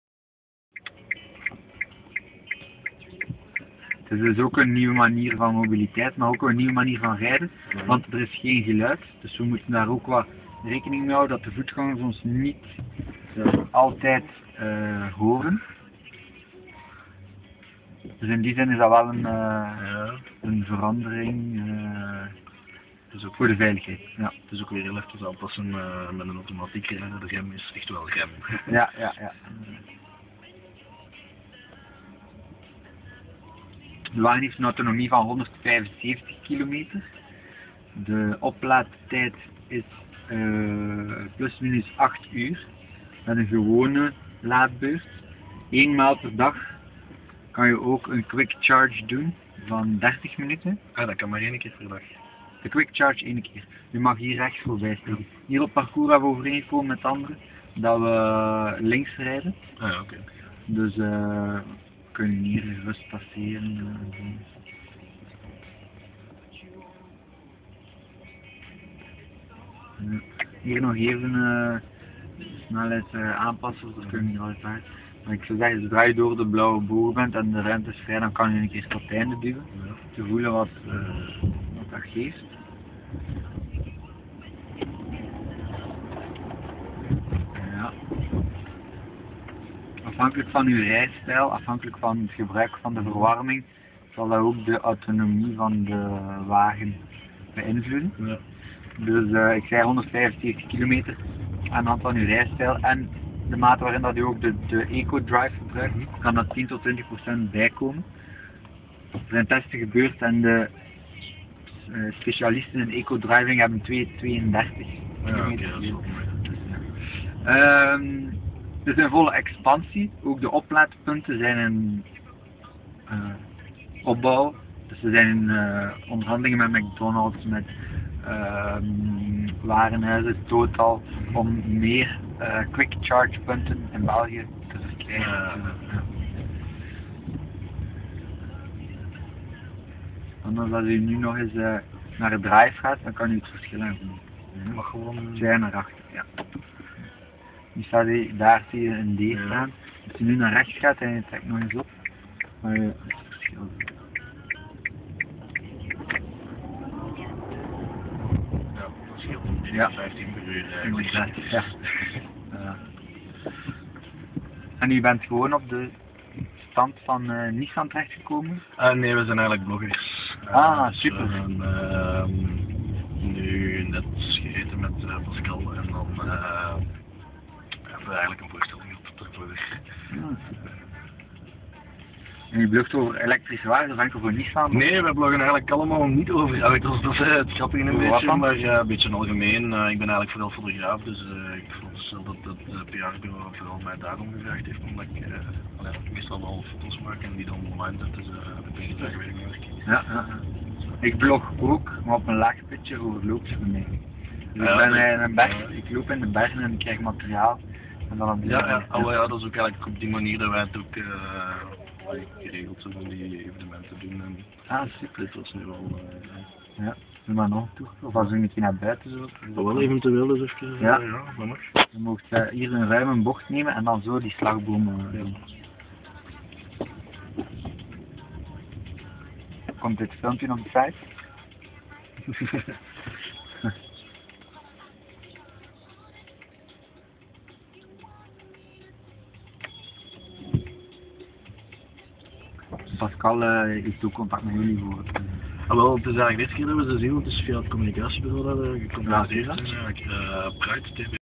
Un circuit en Nissan Leaf version Plan Langues
La Nissan Leaf au salon de l'auto de Bruxelles 2012, avec l'argumentaire en version néerlandophone